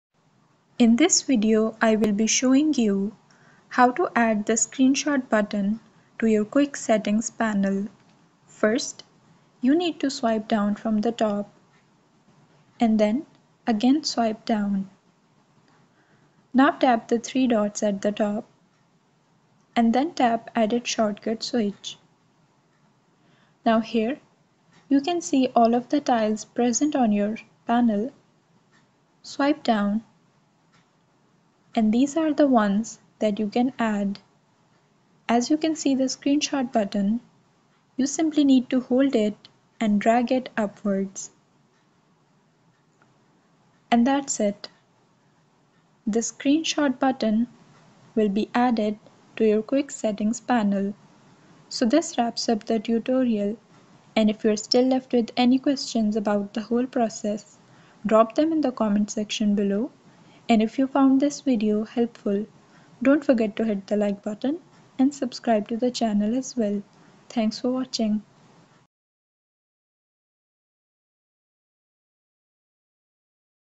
How To Add A Take Screenshot Button To The Quick Settings Panel On Your Phone
Looking for a quicker way to capture your screen?

In this tutorial, I’ll show you how to add a screenshot button to the quick settings panel on your phone. With this simple trick, you can take screenshots instantly without having to press multiple buttons.

This step-by-step guide will cover:

- How to find and customize your quick settings panel
- How to add the screenshot toggle for easy access
- Tips for taking screenshots more efficiently

Whether you’re using an Android device or another smartphone, this method will save you time and make taking screenshots much easier. It’s perfect for anyone who wants quick access to the screenshot feature!